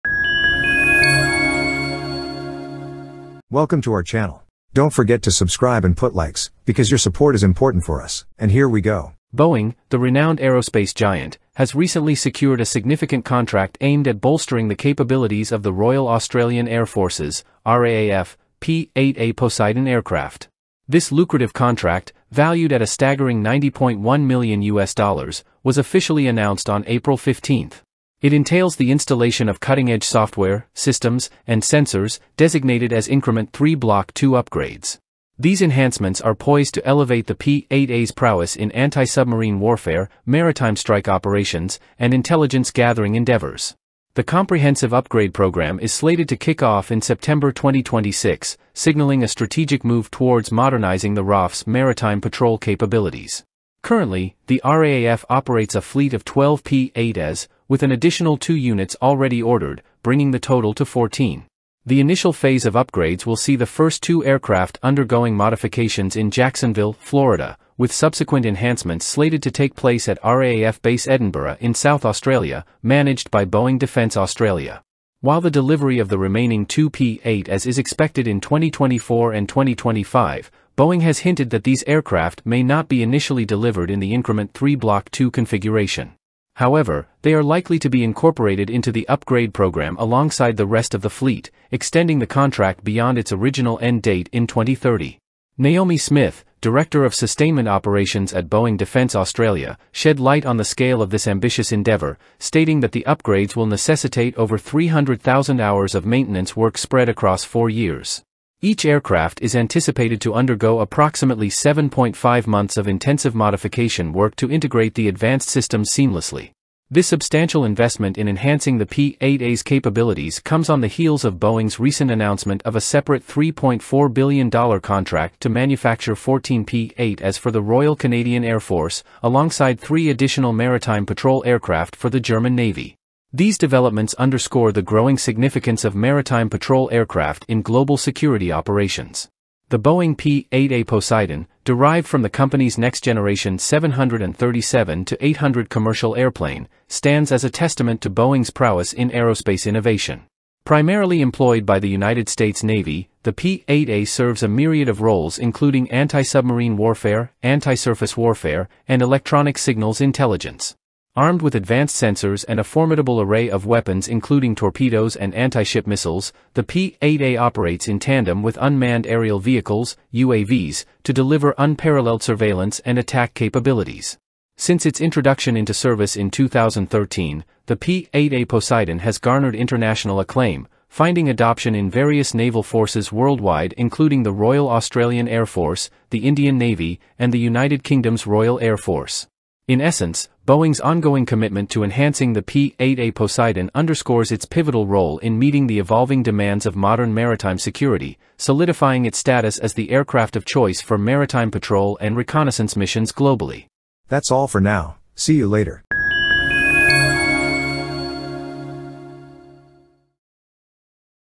Boeing Secures Contract for Australian P 8A Poseidon Upgrades
Boeing, the renowned aerospace giant, has recently secured a significant contract aimed at bolstering the capabilities of the Royal Australian Air Force's (RAAF) P-8A Poseidon aircraft. This lucrative contract, valued at a staggering USD 90.1 million, was officially announced on April 15th. It entails the installation of cutting-edge software, systems, and sensors, designated as Increment 3 Block 2 upgrades. These enhancements are poised to elevate the P-8A's prowess in anti-submarine warfare, maritime strike operations, and intelligence gathering endeavors.